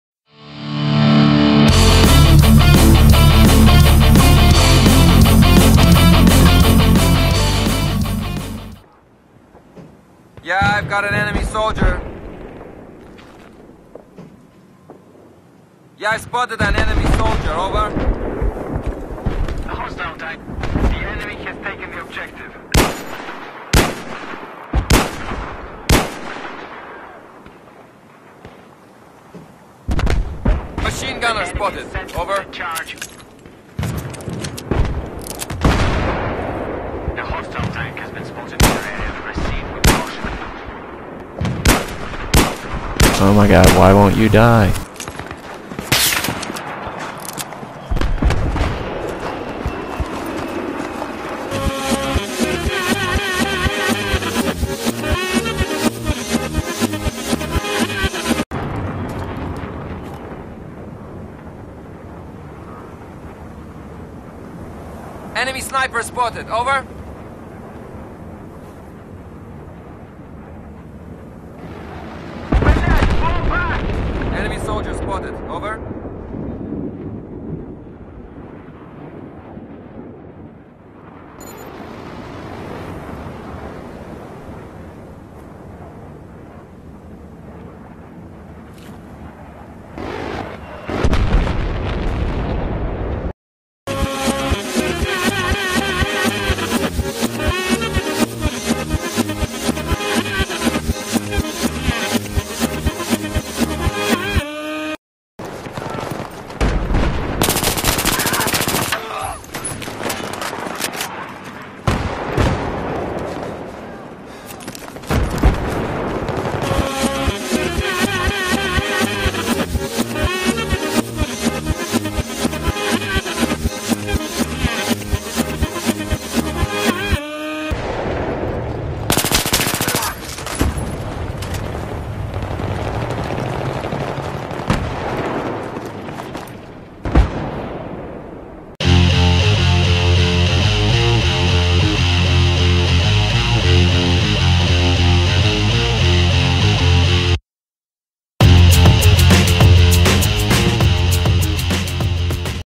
Kill of the Day 10 Jun 2012
I try getting this guy back in enemy territory, but he won't die. Then I chase after him and get him not once but twice. Yeah!!!

Today's Victim = MyopicHorizon